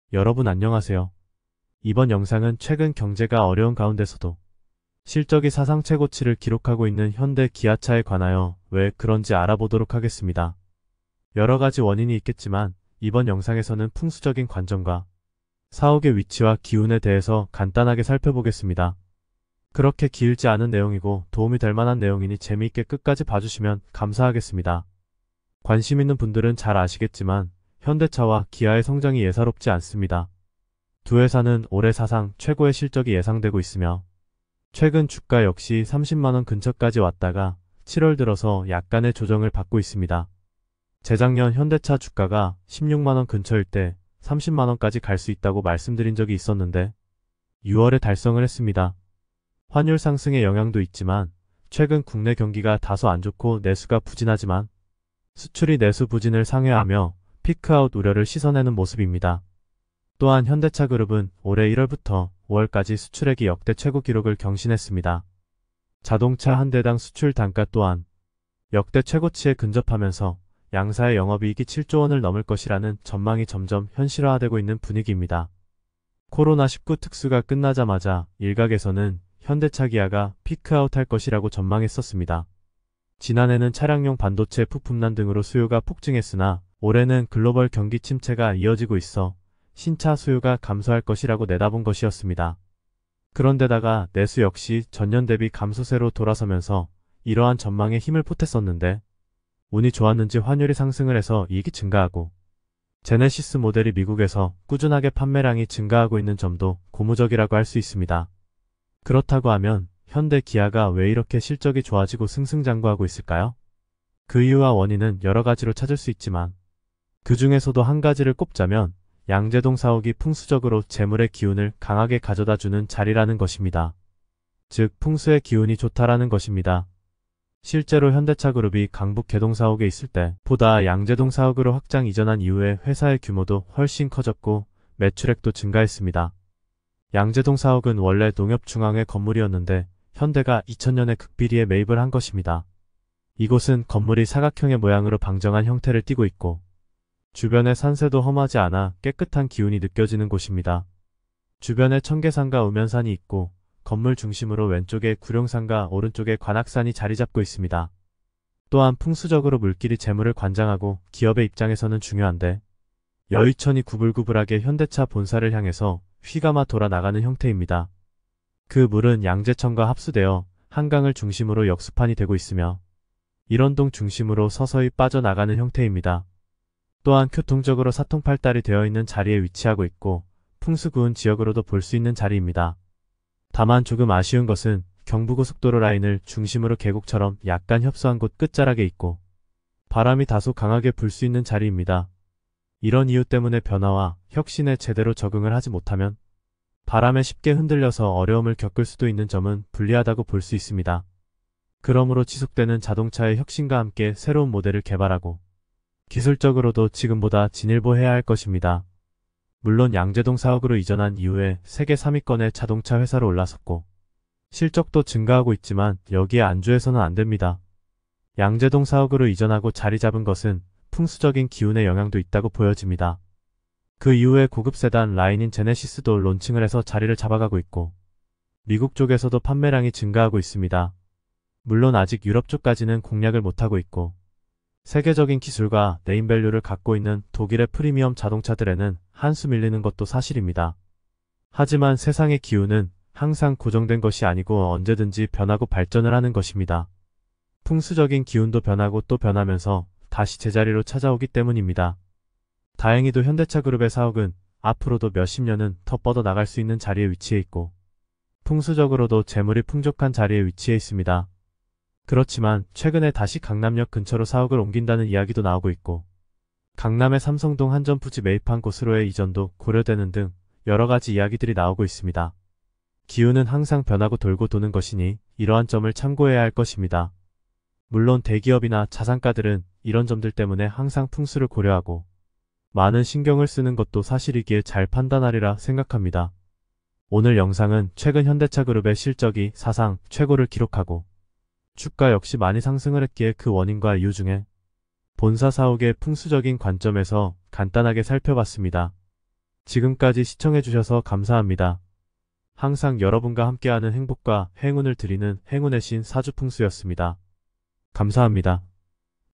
세계3위 자동차 회사로 우뚝 올라선 현대 기아차의 본사 사옥은 풍수적으로 재물(財物)이 쏟아지는 돈복이 넘치는 자리 이기 때문인 이유
현대 기아차의 풍수적인 기운과 앞으로 운기를 알아 봅니다.